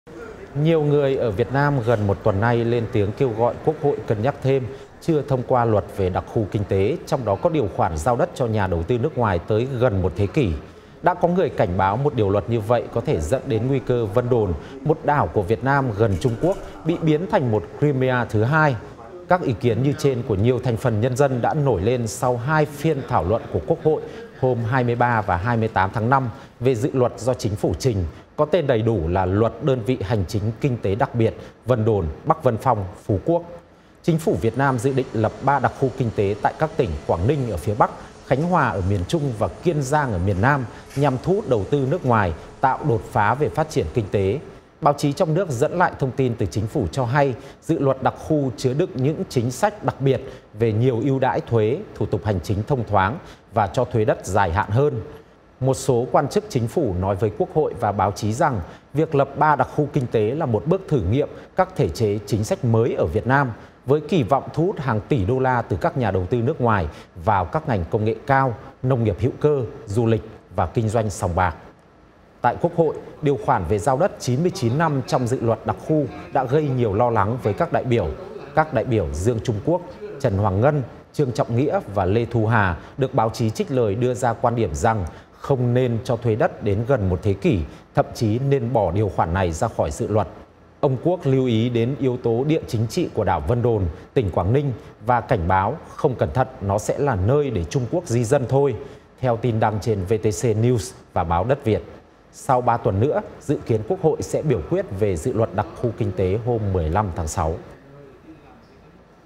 Lo ngại Vân Đồn thành Crimea thứ hai (VOA)
Tin tức Nếu không vào được VOA, xin các bạn hãy vào để vượt tường lửa. Nhiều người ở Việt Nam gần một tuần nay lên tiếng kêu gọi quốc hội cân nhắc thêm, đừng vội thông qua luật về đặc khu kinh tế, trong đó có điều khoản giao đất cho nhà đầu tư nước ngoài tới gần 1 thế kỷ.

Đã có người cảnh báo một điều luật như vậy có thể dẫn đến nguy cơ Vân Đồn, một đảo của Việt Nam gần Trung Quốc, bị biến thành một Crimea thứ hai.

Các ý kiến đó của nhiều thành phần nhân dân đã nổi lên sau hai phiên thảo luận của quốc hội hôm 23 và 28/5 về dự luật do chính phủ trình, có tên đầy đủ là Luật Đơn vị hành chính-kinh tế đặc biệt Vân Đồn, Bắc Vân Phong, Phú Quốc.

Chính phủ Việt Nam dự định lập 3 đặc khu tại các tỉnh Quảng Ninh ở miền bắc, Khánh Hòa ở miền trung và Kiên Giang ở miền nam nhằm thu hút đầu tư nước ngoài, tạo đột phá về phát triển kinh tế.

Báo chí trong nước dẫn lại thông tin từ chính phủ cho hay dự luật đặc khu chứa đựng các chính sách đặc biệt về nhiều ưu đãi thuế, thủ tục hành chính thông thoáng và cho thuê đất dài hạn hơn.

Một số quan chức chính phủ nói với quốc hội và báo chí rằng việc lập 3 đặc khu là một bước “thử nghiệm” các thể chế, chính sách mới ở Việt Nam, với kỳ vọng thu hút hàng tỉ đôla từ các nhà đầu tư nước ngoài vào các ngành công nghệ cao, nông nghiệp hữu cơ, du lịch, và kinh doanh sòng bạc (casino).

Giới hoạch định chính sách bày tỏ hy vọng rằng các đặc khu sẽ có mức thịnh vượng vượt trội nhờ các ưu đãi, từ đó tạo “tác động lan tỏa, tích cực” tới sự phát triển kinh tế-xã hội của Việt Nam nói chung, theo các báo.

Tại quốc hội, điều khoản về giao đất 99 năm trong dự luật đặc khu đã gây nhiều lo lắng cho các đại biểu.

Các đại biểu Dương Trung Quốc, Trần Hoàng Ngân, Trương Trọng Nghĩa, và Lê Thu Hà được báo chí trích lời đưa ra quan điểm rằng không nên cho thuê đất đến gần 1 thế kỷ, thậm chí nên bỏ điều khoản này ra khỏi dự luật.

Ông Dương Trung Quốc lưu ý đến yếu tố địa chính trị của đảo Vân Đồn, tỉnh Quảng Ninh, và cảnh báo “không cẩn thận nó sẽ là nơi để [Trung Quốc] di dân thôi”, theo tin đăng trên VTC News và báo Đất Việt.

Tuy các đặc khu kinh tế với ưu đãi về đất đai không phải là một mô hình mới, với thực tế là đã nhiều nước trên thế giới thực hiện các đại dự án kiểu như vậy, song tính nhạy cảm về vấn đề này ở Việt Nam có phần nguyên nhân ở những nghi ngại của người Việt về những động thái của Trung Quốc trong cả quá khứ lẫn hiện tại. Tiến sĩ Trần Công Trục, nguyên Trưởng ban Biên giới Chính phủ, giải thích với VOA:

“Với thời hạn đó quá dài, sợ rằng rất nhiều người Trung Quốc, thông qua các nhà đầu tư chẳng hạn, họ sẽ biến trở thành lãnh thổ của họ trên đất Việt Nam. Đấy là một tâm lý do hậu quả một quá trình thực hiện các chính sách của Trung Quốc trong vấn đề biên giới, lãnh thổ, đặc biệt trên Biển Đông”.

Sau khi các ý kiến của các đại biểu quốc hội được báo chí đăng tải, trong nhiều ngày liên tiếp, đông đảo dư luận Việt Nam, bao gồm các thành phần đa dạng như các nhà báo kỳ cựu, giảng viên đại học, quan chức về hưu và các nhà hoạt động, cũng bày tỏ ý kiến trên mạng xã hội phản đối dự thảo về giao đất lâu gấp rưỡi thời hạn theo luật hiện hành.

Trong một bài đăng trên Facebook cá nhân được nhiều người chia sẻ, cũng như được trang mạng có tên Báo Tiếng Dân đăng lại, tiến sĩ Võ Trí Hảo nói ông quan ngại nhất về nguy cơ đối với Vân Đồn do đảo này có “giá trị quốc phòng” đối với Trung Quốc.

Vị tiến sĩ nhắc lại đặc điểm của hòn đảo là “cận kề Trung Quốc, có lịch sử sinh sống của người Trung Quốc trước năm 1979”, thời điểm quan hệ Việt Nam-Trung Quốc trở nên thù địch do nổ ra cuộc chiến biên giới giữa hai nước.

Dự kiến quốc hội sẽ biểu quyết về dự luật đặc khu kinh tế vào ngày 15/6 tới đây.